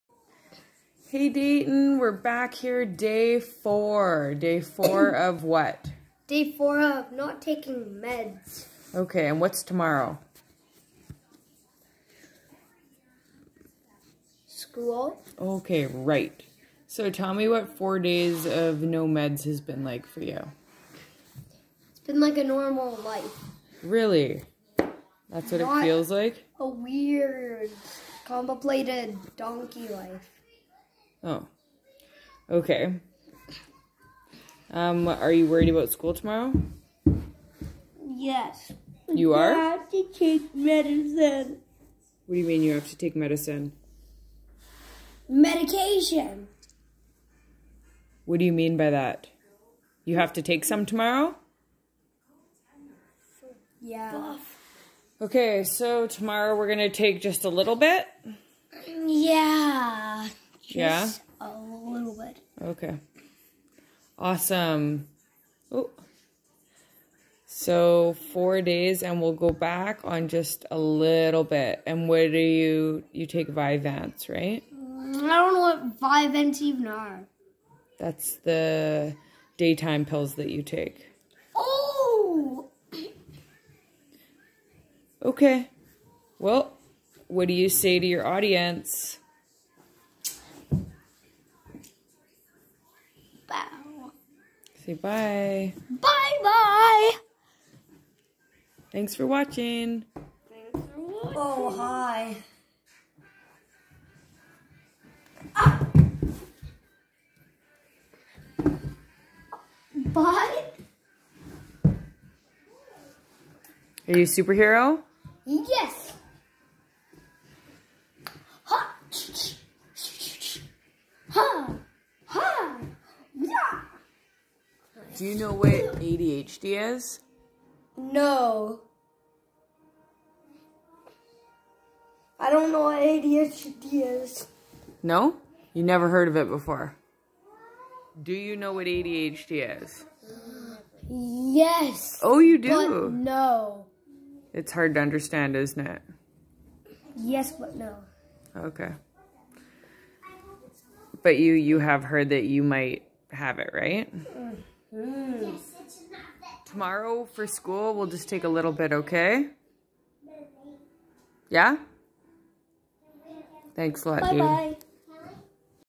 4 days no drugs! Eps. 2
was a bit of a challenging weekend speaking on behalf of us parents. Dayton feels excellent and managed to excel with little reminders to chill. We had a big family gathering for Thanksgiving dinner and that's a lot of stimulation, he did great. Today he had a friend over and was a little over the tip excitable and annoyed his friend. oh well. tomorrow is a new day and we will take his Vyvanse down to half the dose and hope school is OK.

more updates on the journey coming soon.
we speak to the pediatrician on Wednesday and look forward to weaning off fully by Christmas break!

we are an open book, should you have any questions just ask!?